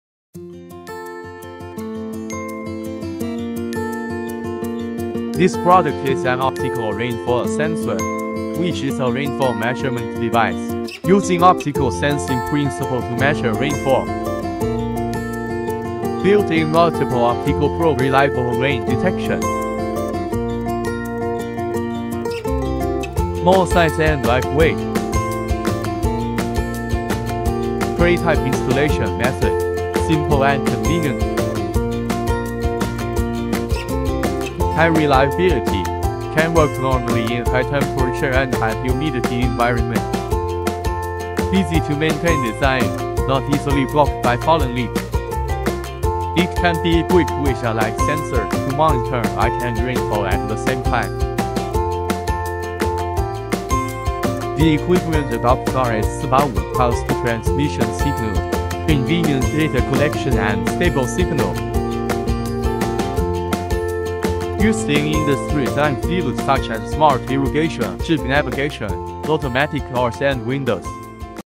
Optical rain gauge from Renke
The Renke optical rain gauge series is a reliable and cost-effective choice for precipitation measurement. The overall size of the optical rain gauge is about the size of a tennis ball, and the internal sensor is completely sealed from the external environment. The delivery comes with mounting brackets that can be easily installed on weather stations, street lights and other infrastructure.